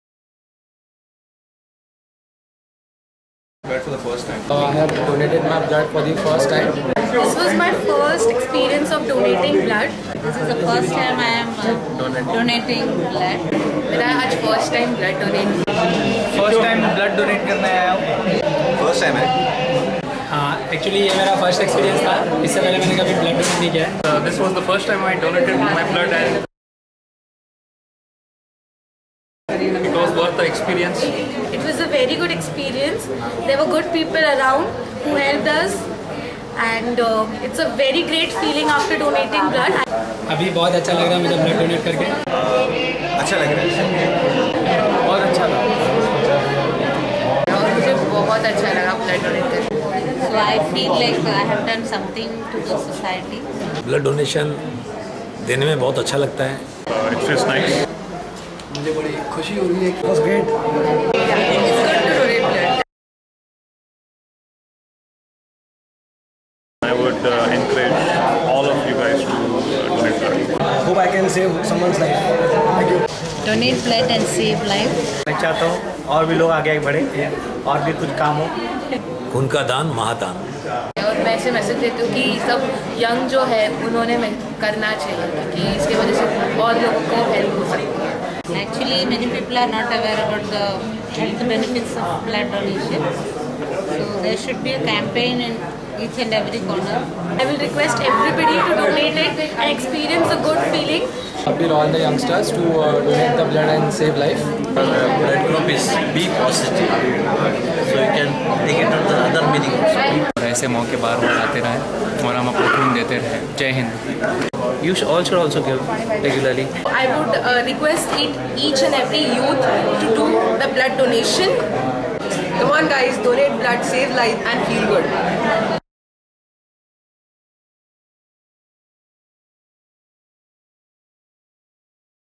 I am not GOD, but saved lives.... Blood Donation Drive on Independence Day
I am not GOD but saved lives. this was the message expressed by the blood donors on the Independence Day at Bangur Nagar, Goregaon (W), Mumbai. Indian Development Foundation in collaboration with Bangur Nagar Mitra Mandal Charitable Trust organised the Blood Donation Drive on 15th Aug. 2012. Event supported by Rotary Club of Mumbai North End, Meenatai Thackeray Blood Bank, We Are Hardevians (WAH), NSS Unit from Vivek College and UPG College of Management.